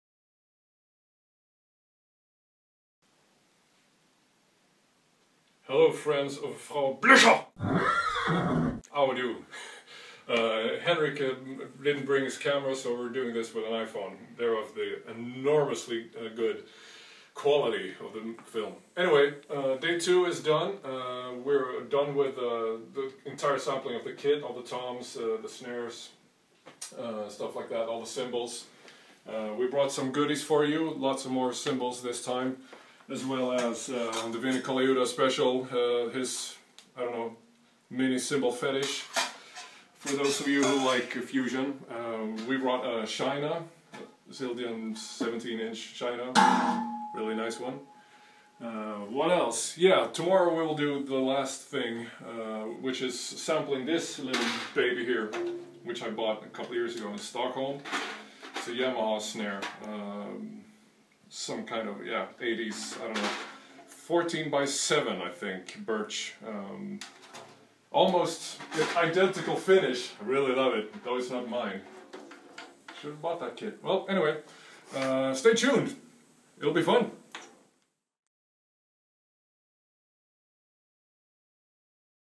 The Slick Kit - Day 2
We are done with the second day of sampling "The Slick Kit". This kit for Native Instruments Kontakt 5 consists of:

Yamaha 9000 Recording Custom kit
20" kick
10", 12", 13" and 15" toms

60's Ludwig Supraphonic snare in two different tunings.
80's Yamaha snare drum in a fat, low tuning.

20" Zildjian Constantinople Medium Thin High Ride
16" A Zildjian Fast Crash
19" Zildjian Kerope Crash/Ride
19" Zildjian A Custom Rezo Crash
16" 70's Zildjian A Crash
17" Zildjian K China
8" Zildjian A Splash
10" Zildjian A Splash
14" Zildjian hihat - New Beat Top/Armand Bottom

Cowbell

For release mid August 2014.